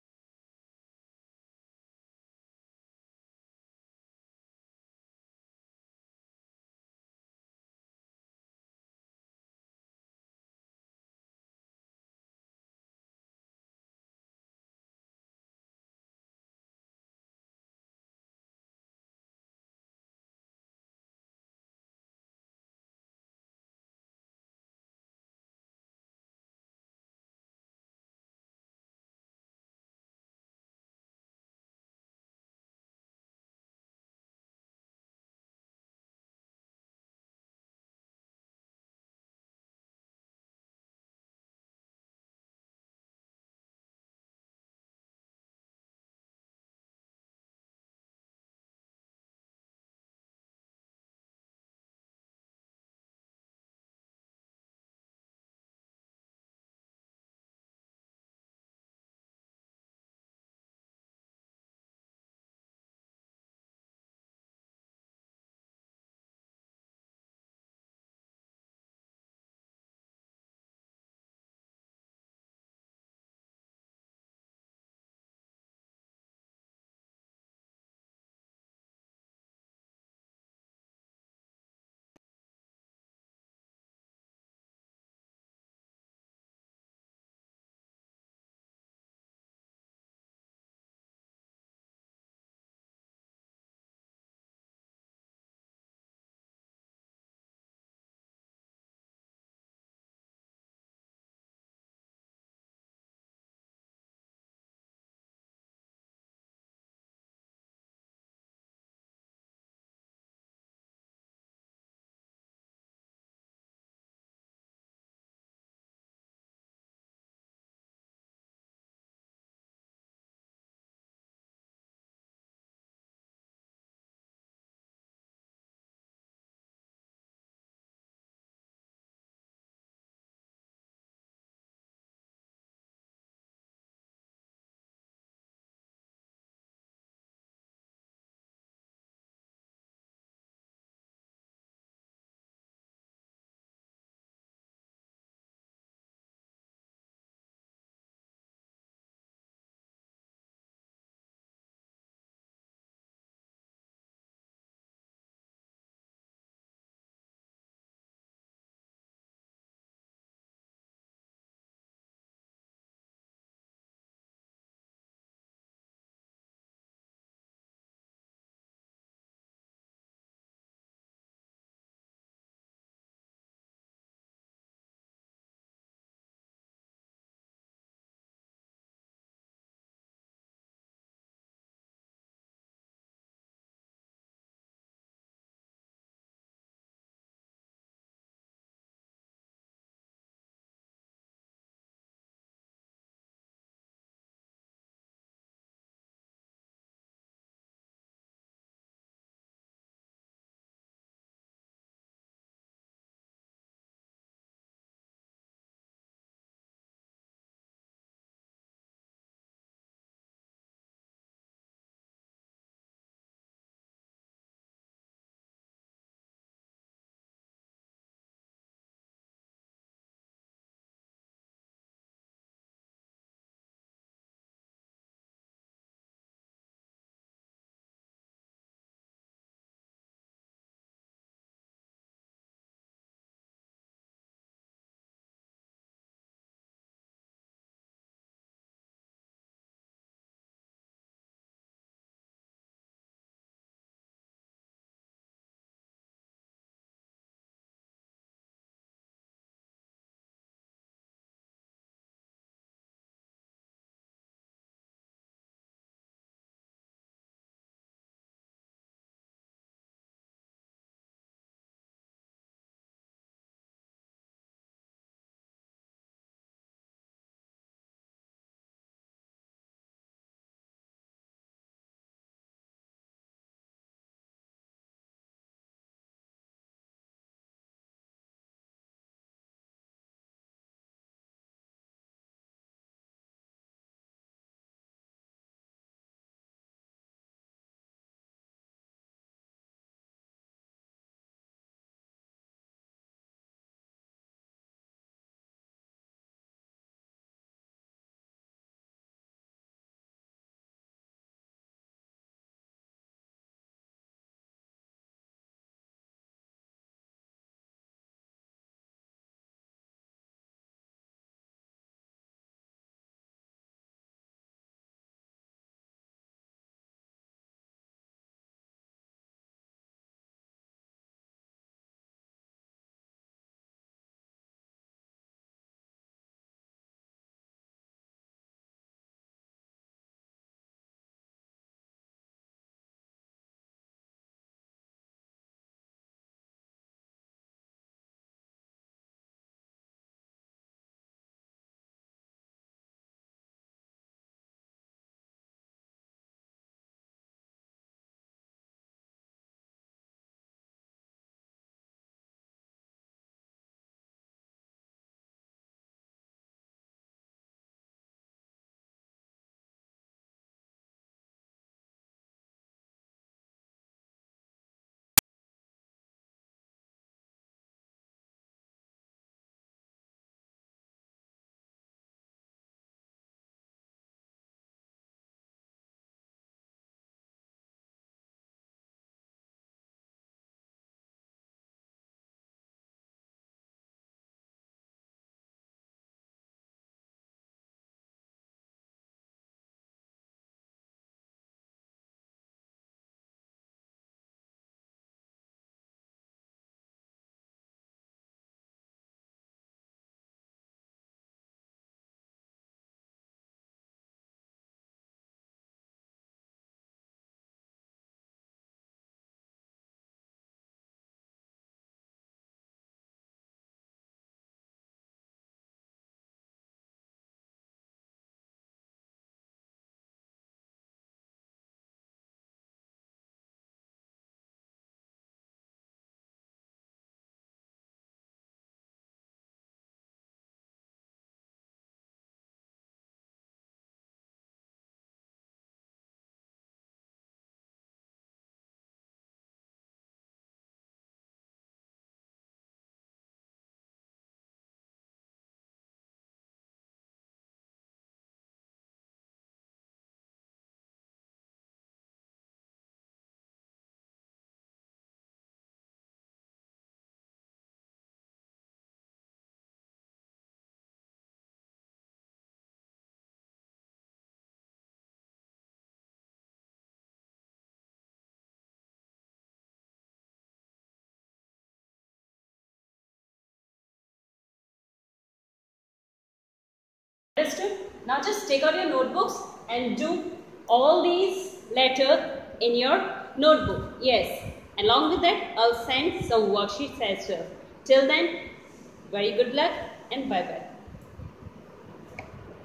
Mrs. Maitra - Nursery Class Teacher,Delhi Public School Makronia
English alphabets - letter Qq along with the revision of previous letters from Aa to Pp.